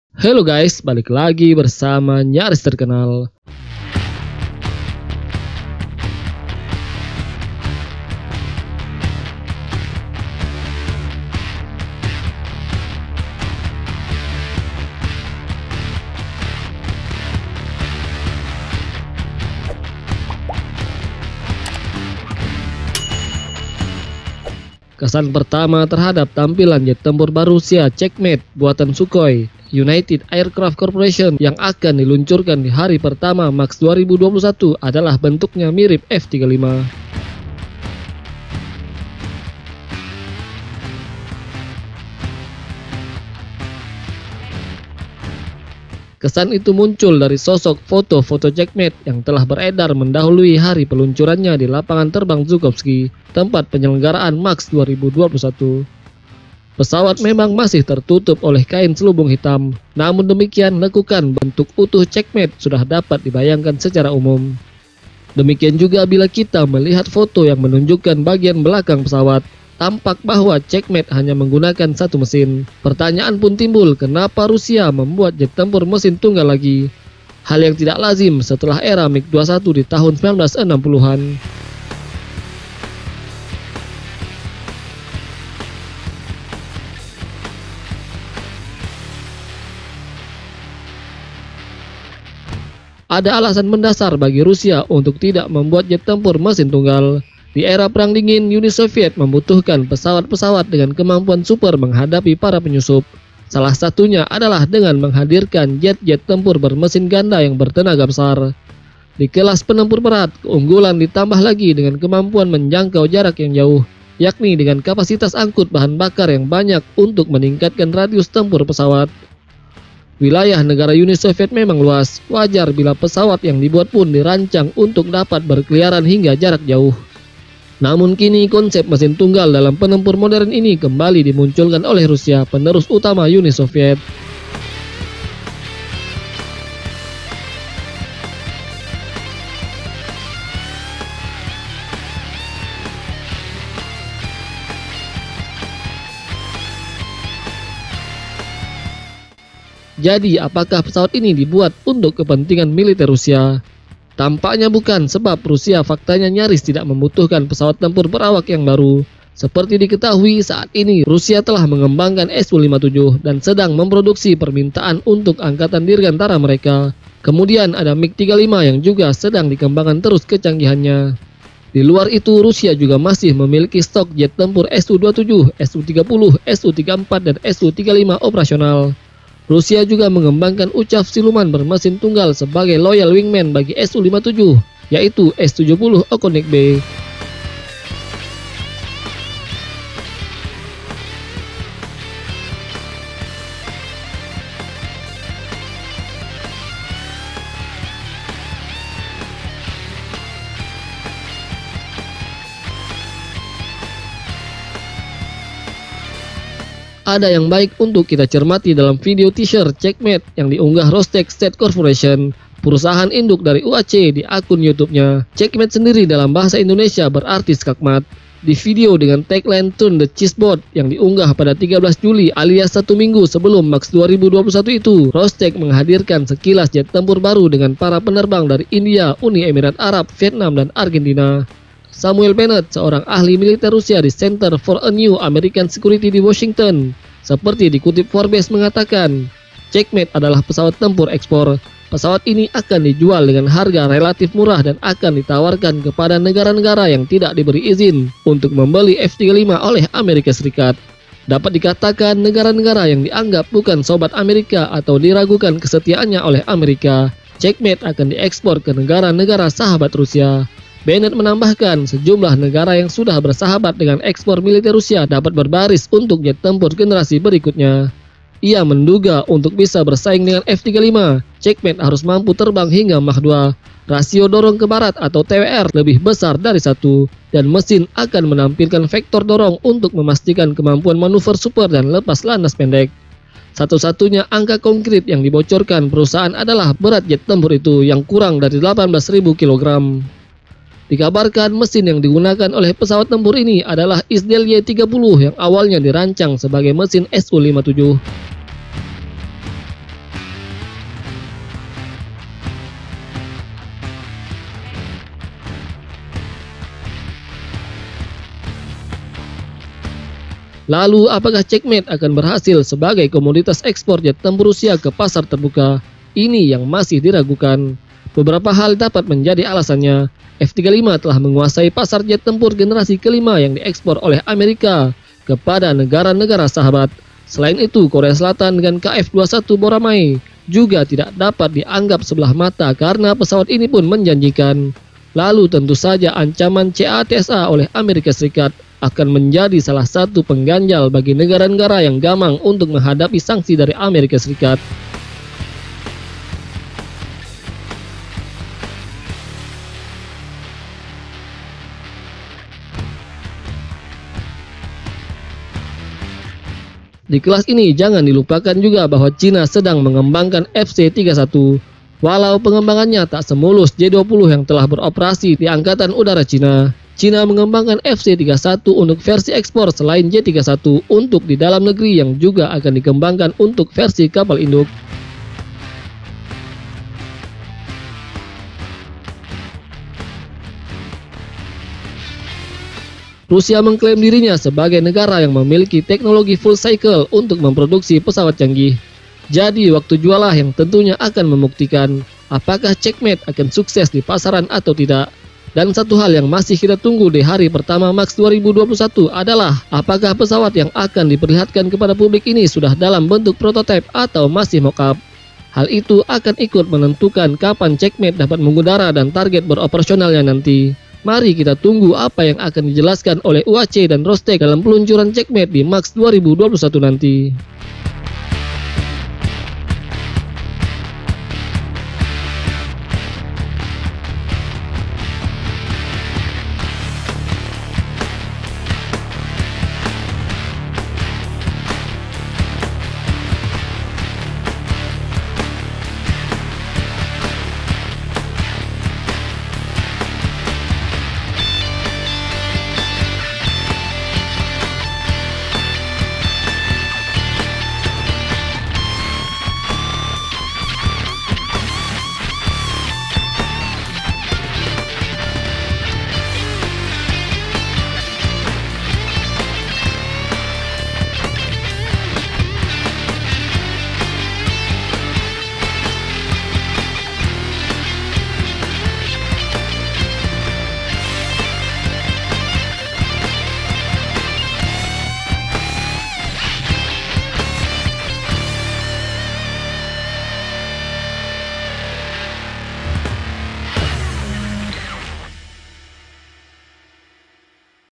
TAK LAZIM! KENAPA TIBA TIBA RUSIA MEMBUAT JET TEMPUR SUKHOI CHEKMATE BERMESIN TUNGGAL LAGI?
Pertanyaan timbul, KENAPA Rusia membuat jet tempur mesin tunggal lagi? Hal yang tidak lazim setelah era MiG-21 di tahun 1960-an.

Ada alasan mendasar bagi Rusia untuk tidak membuat jet tempur mesin tunggal. Di era Perang Dingin Uni Soviet membutuhkan pesawat-pesawat dengan kemampuan super menghadapi para penyusup. Salah satunya adalah dengan menghadirkan jet-jet tempur bermesin ganda yang bertenaga besar.
Di kelas penempur berat, keunggulan ditambah lagi dengan kemampuan menjangkau jarak yang jauh. Yakni dengan kapasitas angkut bahan bakar yang banyak untuk meningkatkan radius tempur pesawat.

Wilayah negara Uni Soviet memang luas. Wajar bila pesawat yang dibuat pun dirancang untuk dapat berkeliaran hingga jarak yang jauh.

Namun kini, konsep mesin tunggal dalam penempur modern ini kembali dimunculkan oleh Rusia, penerus utama Uni Soviet.

JADI Apakah pesawat ini dibuat untuk kepentingan militer Rusia?